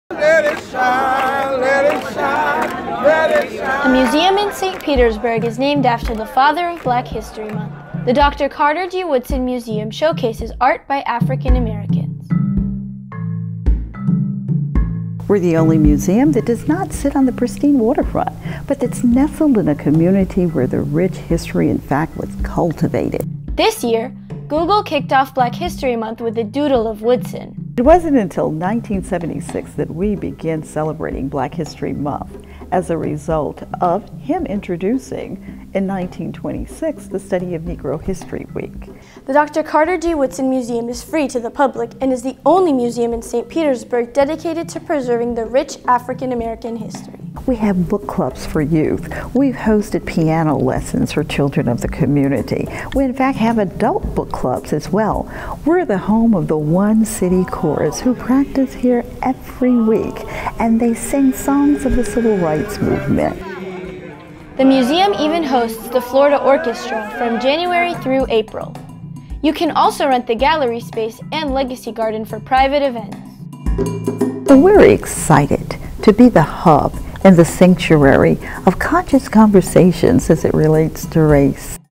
Dr. Carter G. Woodson African American History Museum
By Nada Blassy

ST. PETERSBURG, Fla. – The Dr. Carter G. Woodson African American History Museum is an excellent place to learn about the Father of Black History Month, as well as the African American culture in the Bay area.

“We here at the Dr. Carter G Woodson African American History Museum take delight in not only preserving, presenting but interpreting African American history,” said Terri Lipsey Scott, executive director of the museum. “We celebrate the contributions of those past, but more importantly those of our community of current.”

The museum features bits of information about Dr. Woodson, but a fact not in the museum is that Dr. Woodson was selected as, “the doodle for Google.” Scott continued by saying that, “his popularity is growing, particularly with this generation.”

“It wasn't until 1976, that we begin celebrating Black History Month as a result of him introducing in 1926, the study of Negro history week,” said Scott.

The Dr. Carter G. Woodson African American History Museum is the only museum in St.Petersburg dedicated to African American history. What makes this museum different than others is, “We’re the only museum that does not sit on sit on the pristine waterfront, but that’s nestled in a community where the rich history was in fact cultivated,” said Scott.

The museum is free to the public, but the staff kindly requests donations.

“We take pride in being able to showcase the talented work of so many artists throughout the Bay area and beyond,” said Scott.
“The Dr. Carter G Woodson African American History Museum, in fact, hosts every six to eight weeks a new exhibit. Because of the limited space that we have here,” Scott continued, “We are prideful in order to just showcase the talented art of African American artists who have never been seen or shown anywhere else, locally, nationally, or internationally and beyond.”

Many art pieces do not get the opportunity to be showcased and are often stored away in garages. The museum provides a forum for artists to get a chance to display their art.

The museum also hosts a number of activities, such as book clubs and piano lessons. “We’re the home of the One City Chorus,” Scott said, “Who practice here every week, and they sing songs of the Civil Rights Movement.”

On Feb. 1, 2018, St. Petersburg’s City Hall held a flag ceremony honoring the Woodson flag. The flag was raised at 10 a.m., marking the beginning of Black History Month. Across the nation, the city of St. Petersburg is the only place that raises the Woodson flag on a government entity.

In addition to the One City Chorus, the museum partners with The Florida Orchestra, and once a month from January through April, the museum hosts one of their segments.

The grounds of the museum used to be a community center, Jordan Park. In the early 2000s, the space was renovated. Behind the museum is a Legacy Garden. It features bricks with donors’ names on them. The garden is an ongoing fundraiser for the museum.

Scott is, “delighted that folks are embracing not only the culture of African American history, but looking back, and recognizing and in fact celebrating the individual who in fact brought it to the forefront.”